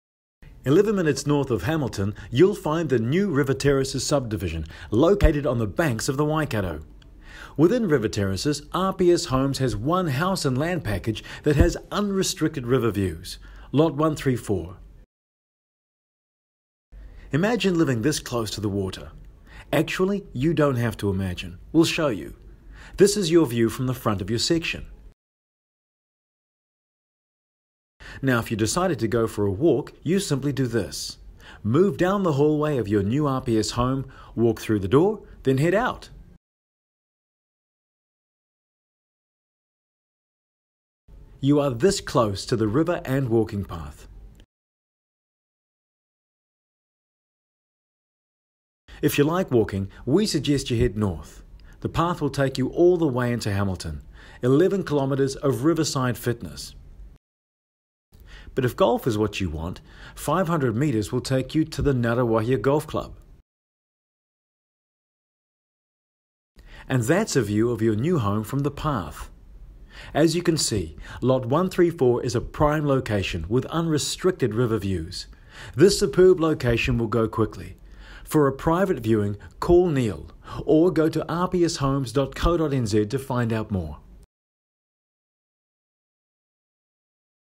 LOT 134 River Terrace VIDEO
NEW Waterfront Home in River Terraces, North Hamilton.